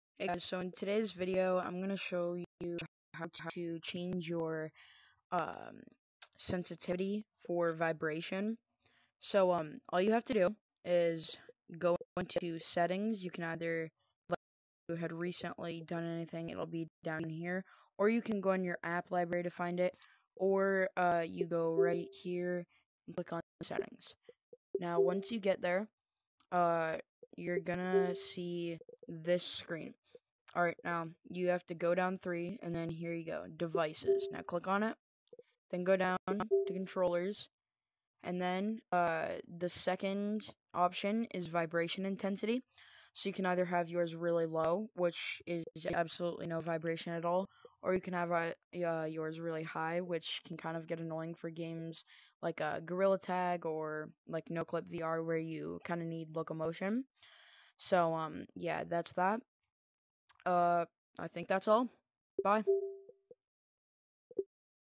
How to change your controller sensitivity on Oculus Quest 2.
This is a video on how to change your controller sensitivity on your Oculus Quest 2.